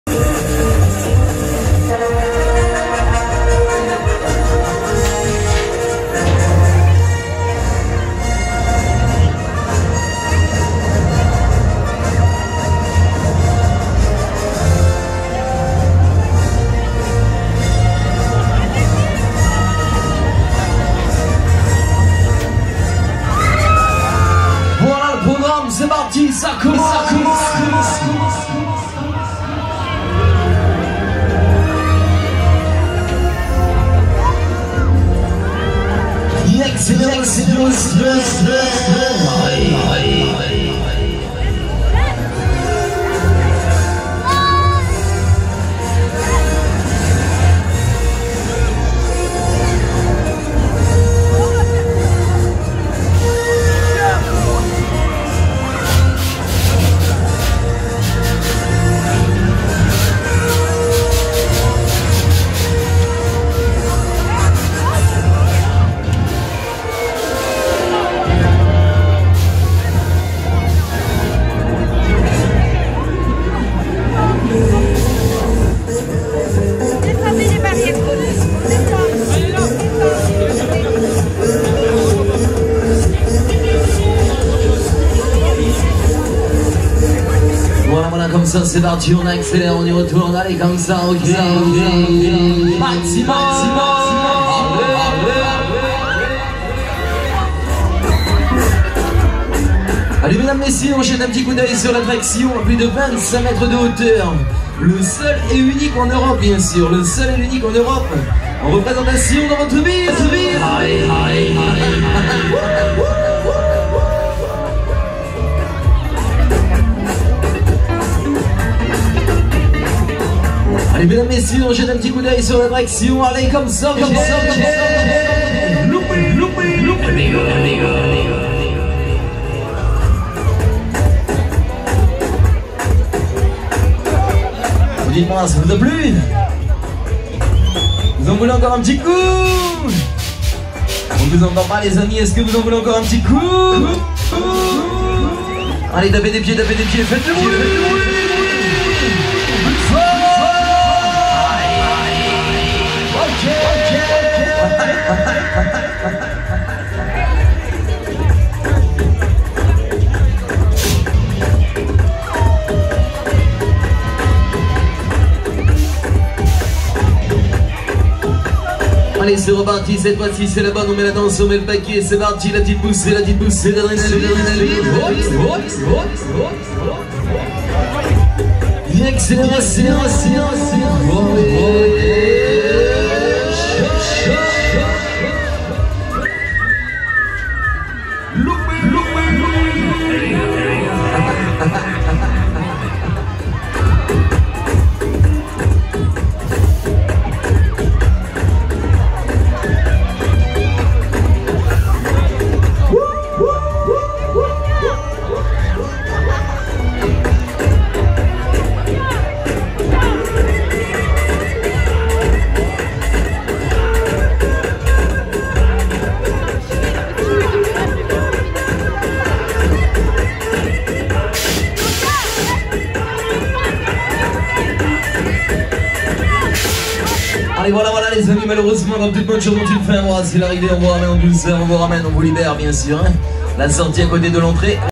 Star Wars (Baillet) - Foire de Valenciennes 2019
Le Star Wars de la famille Baillet, nouveauté sur la foire de Valenciennes édition 2019.